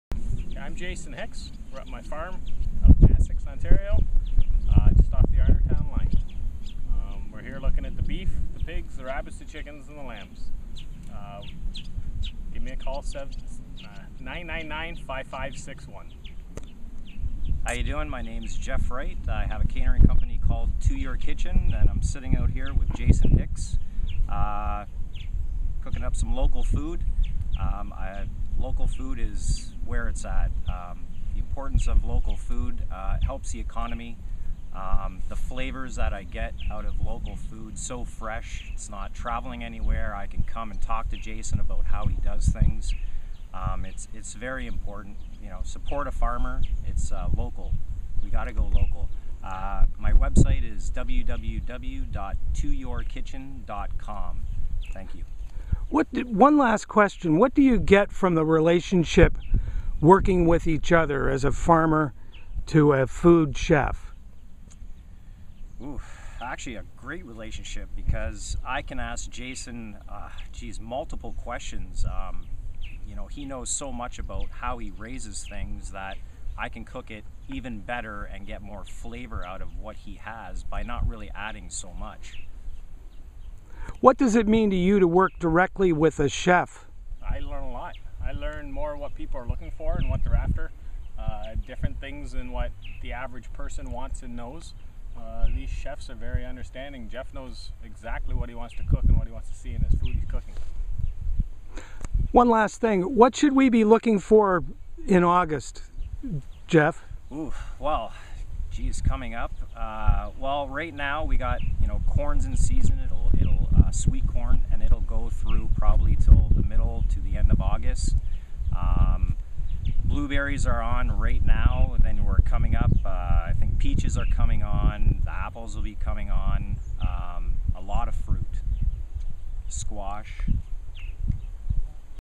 Support A Farmer
Essex-area farmer Jason Hicks and Harrow Chef Jeff Wright talk about working together to provide more local food to area consumers. They're shown at Hicks' farm where he raises Highland cattle
and pigs, poultry and rabbits.